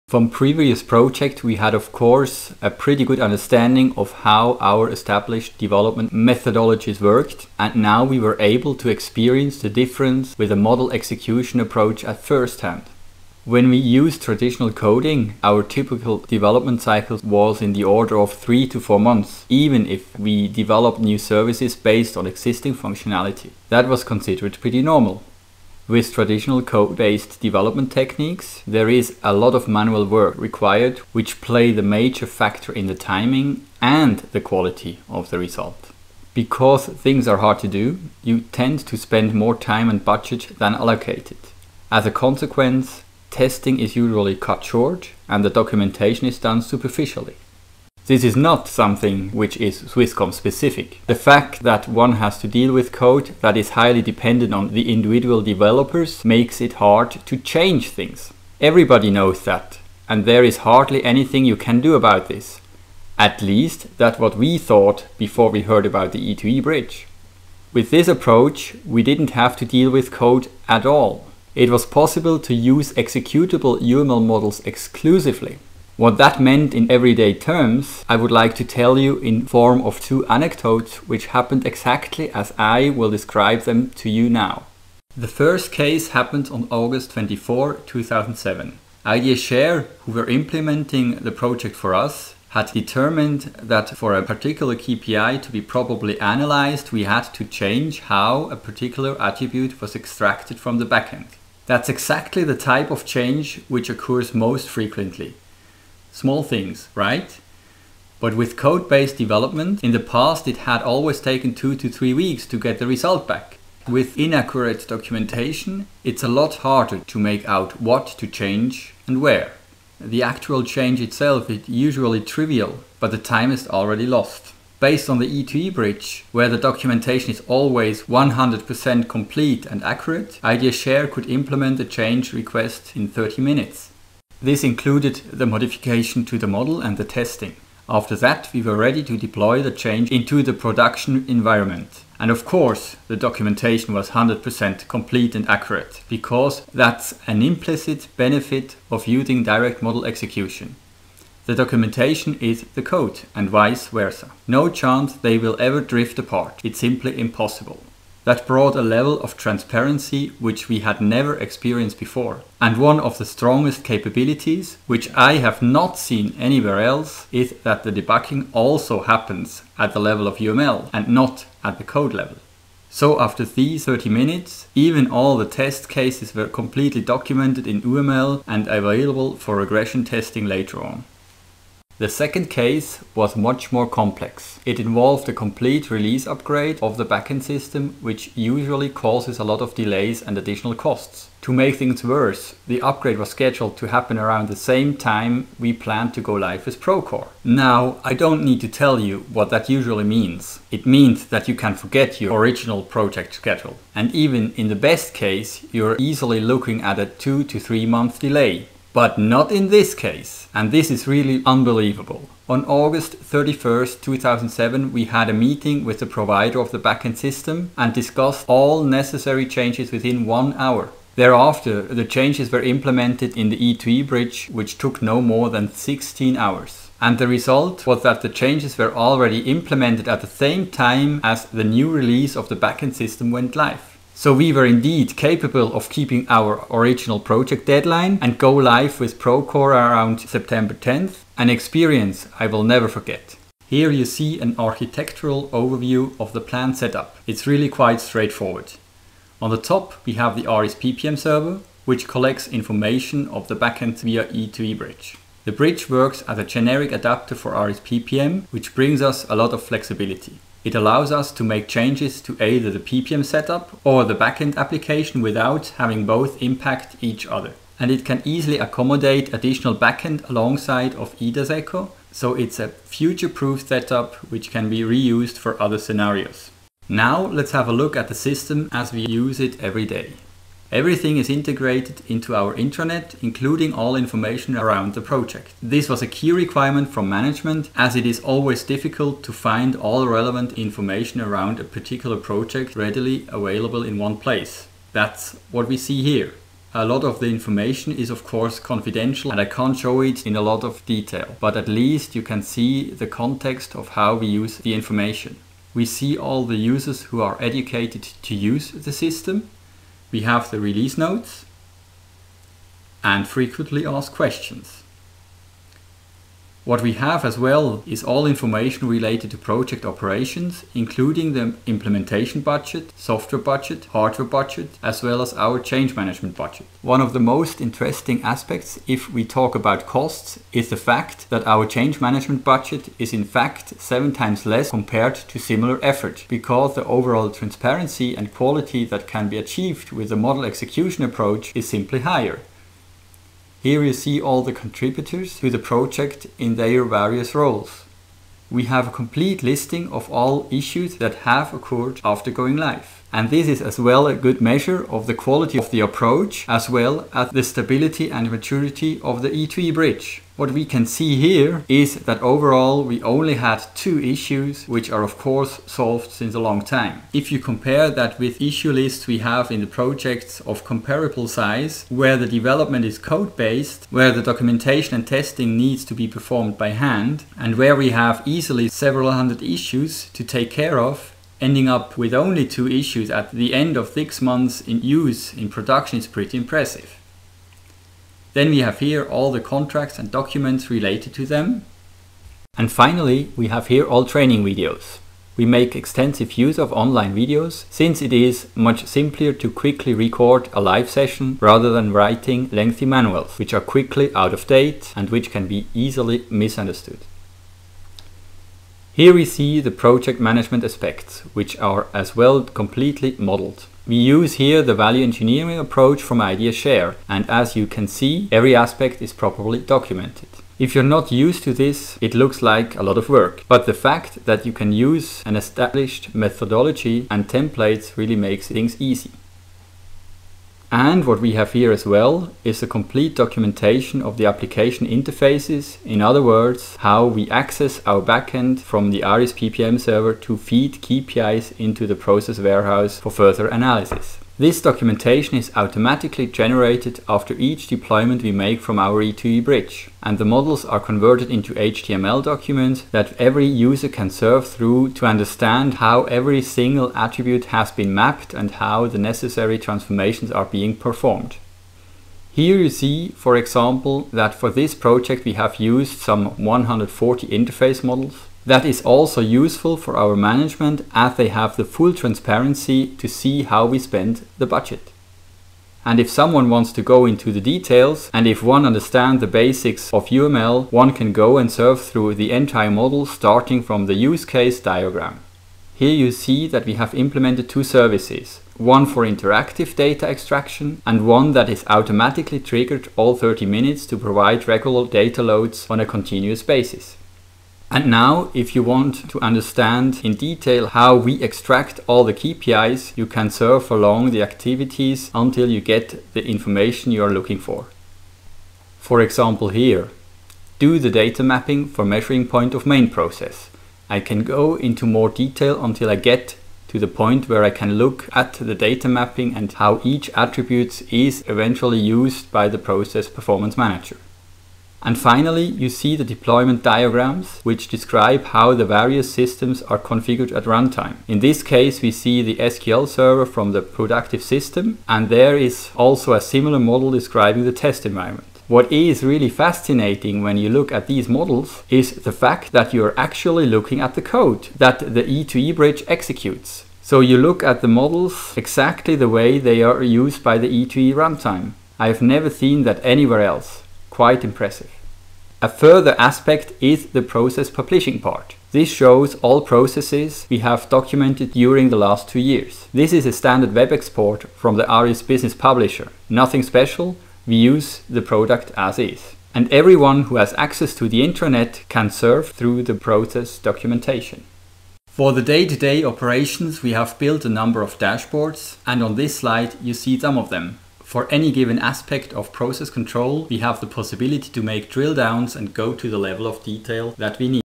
Process Performance With ARIS PPM & E2E Bridge [2,3]
Case Study: "ProCore - Process Performance Management - From Idea To Implementation", an interview with Markus Witschi, Operations Manager SMESwisscom (Schweiz) AG, featuring use of the E2E Bridge as generic interface to extract KPIs from backends for ARIS Process Performance Manager.

Highlights: Model execution leads to transparency & control - Swisscom (Schweiz) AG increases business agility with process monitoring via ARIS PPM, using the E2E Bridge® as a generic interface for real-time KPI extraction from custom backend application.

Challenge: For its SME business, which covers DSL, VPN and other products, Swisscom uses the custom built eDASECO application, which was never integrated with the central data warehouse. As a result, only rudimentary reporting mechanisms were available, denying Swisscom employees to proactively address issues which affected service quality. To remedy the situation, ARIS PPM from IDS Scheer was installed as the monitoring solution. But the PPM integration proved to be challenging: the traditional integration approach required middleware changes to be scheduled six months in advance, and apart from the implementation, most of the time was consumed by manual testing and writing up-to-date documentation. Each modification of the backend, changes to the process or any updates to the KPIs caused considerable effort.